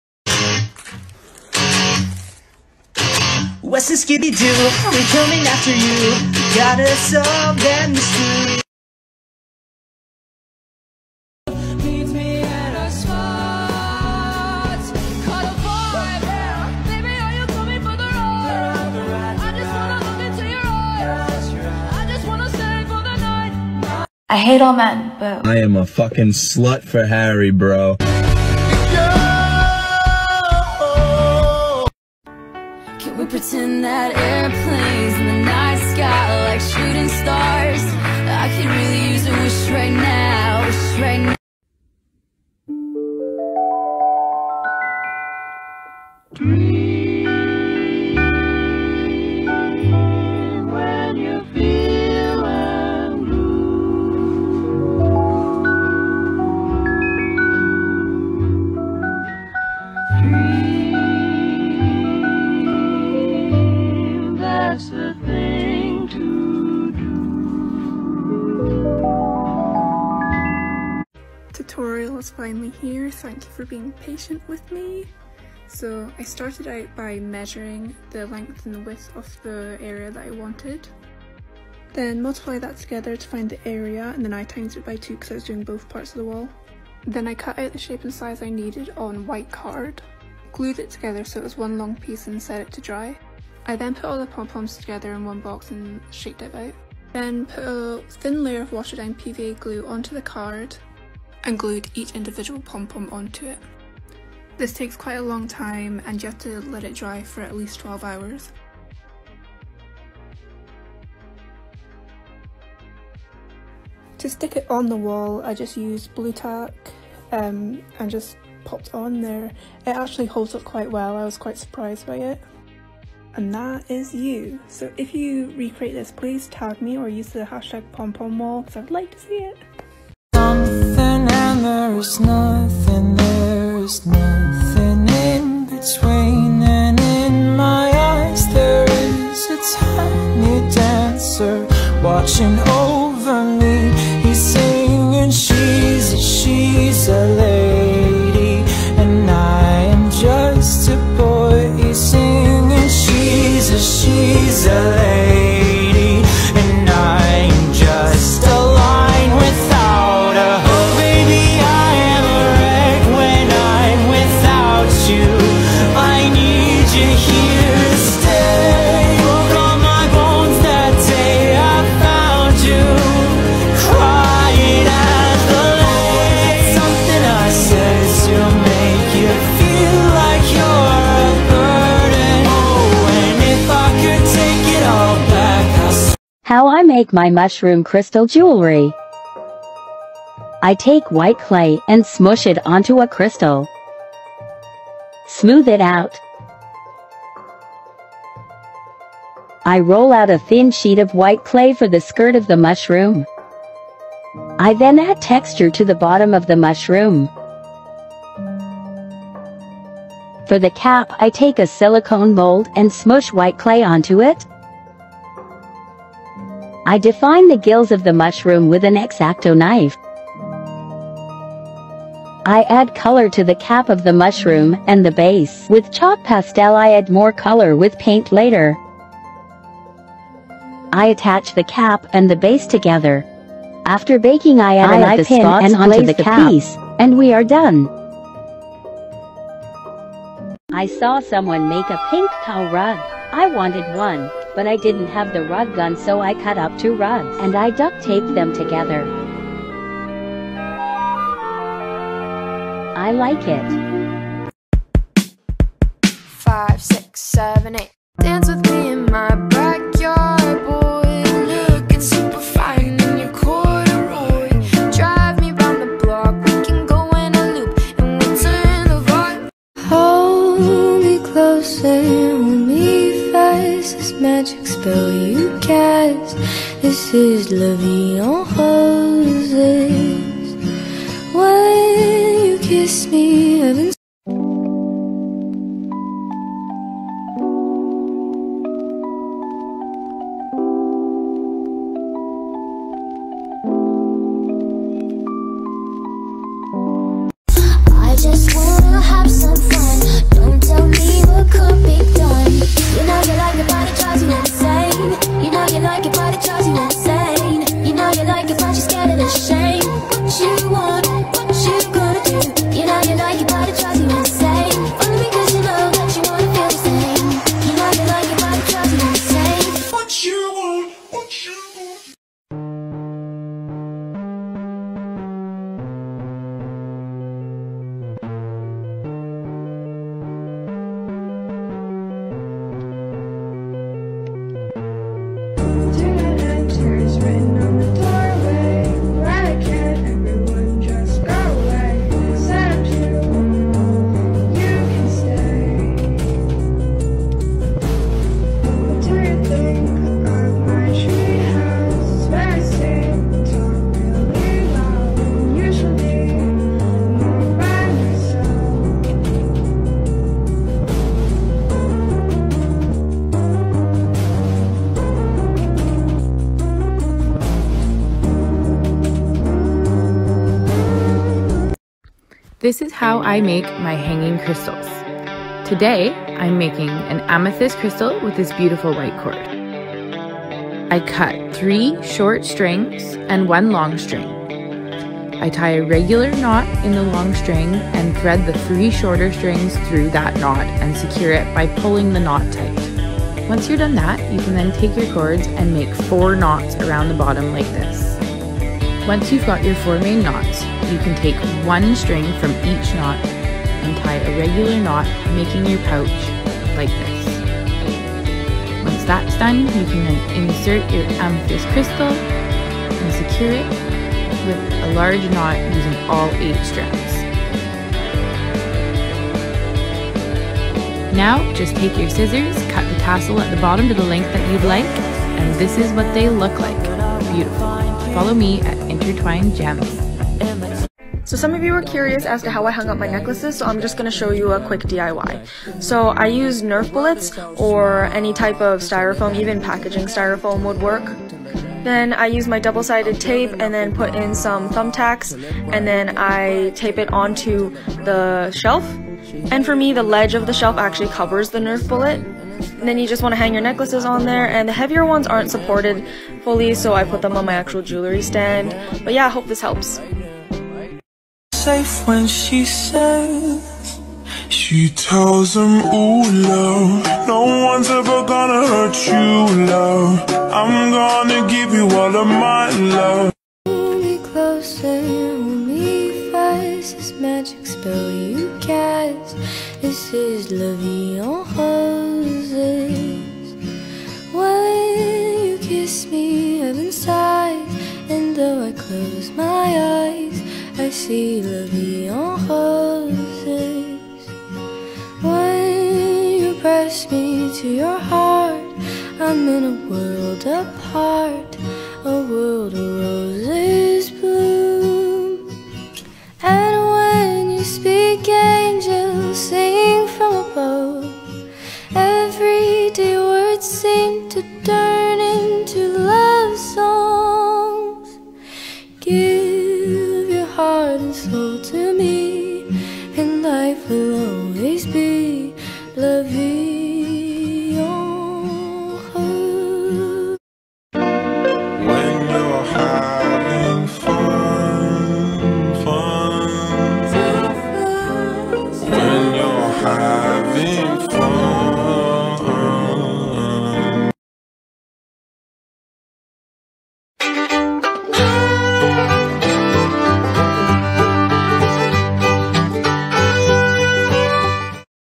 DIY tiktok compilation part 2
Here are some more cute DIY's that I hope you'll like 🥰
I tried to include a variety of things in this compilation so there's some jewelry, some home decor and some other random diy tiktoks.

:) All the creators are credited in the video, please go and support them if you enjoyed their content (: